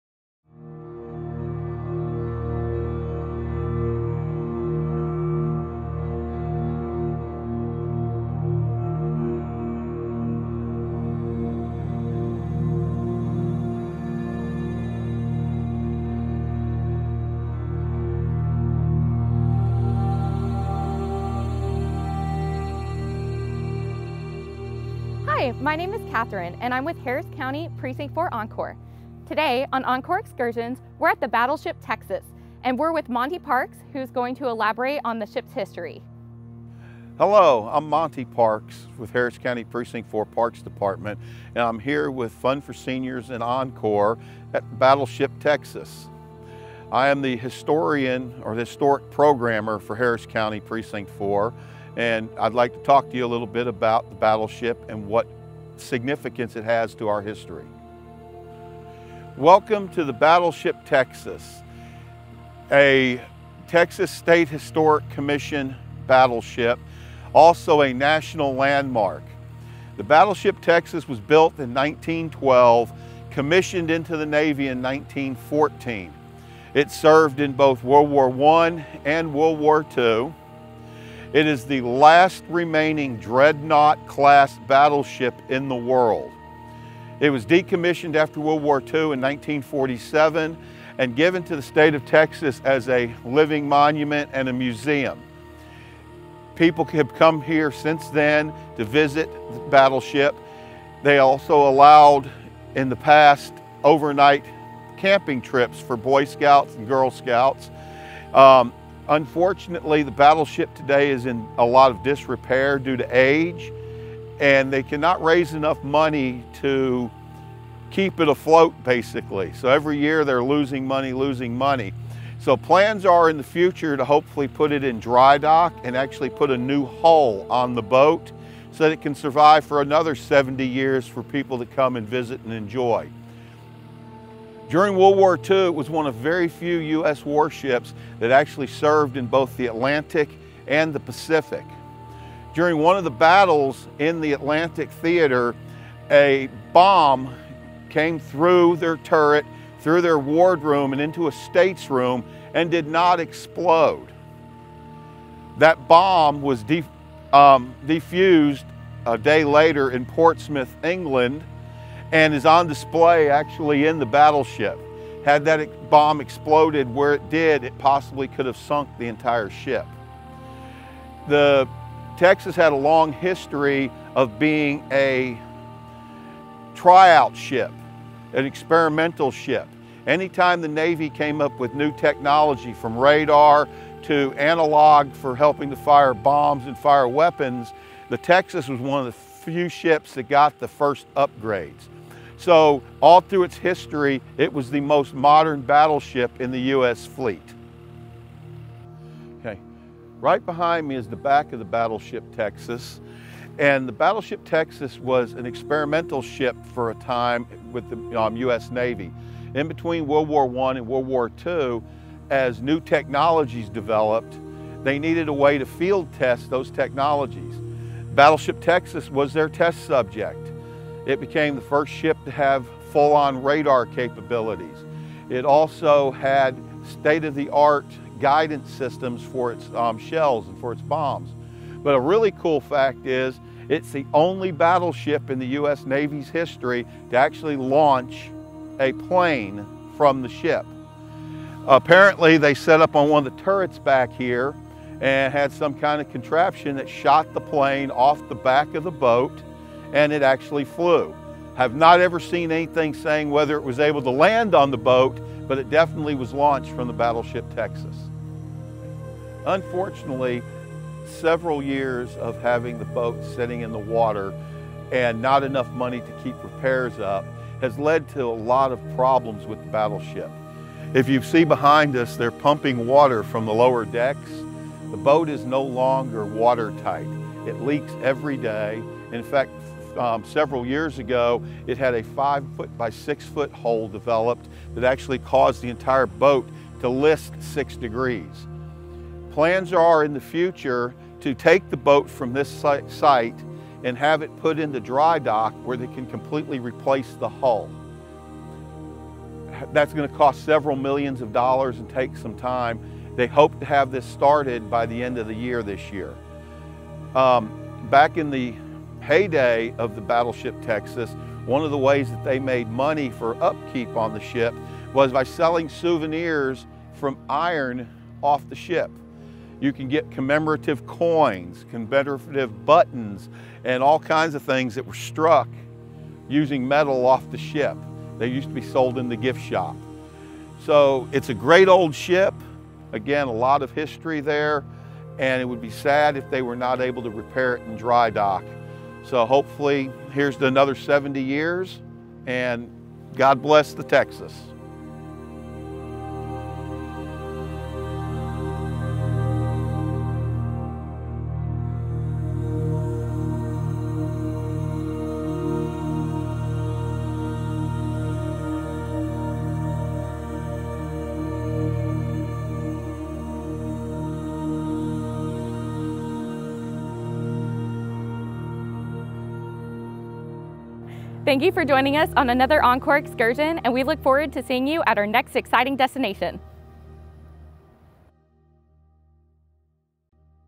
Encore Excursion - Battleship Texas
Join Precinct 4's Monte Parks for this week's Encore! Excursion to the Battleship Texas. Though currently closed for repairs, the Battleship Texas is the first battleship to become a U.S. National Historic Landmark, and also the first battleship to be a permanent museum. Learn more about the history and future plans for this World War II-era ship.